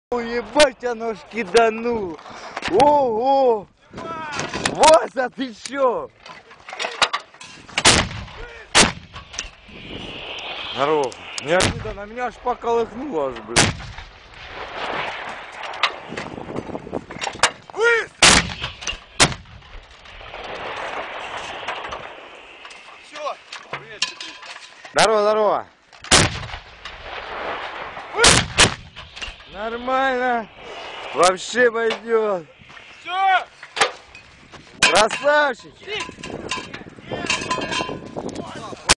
Ukraine War - Russian armed forces shelling Ukrainian army near Donetsk Ukraine
With credits to CHANNEL 6
The present material selected, adapted & published by EMPR #1 independent citizen media about Ukraine | Russian Terrorists Shell Ukrainian Army Positions During Ceasefire, Feb 23 2015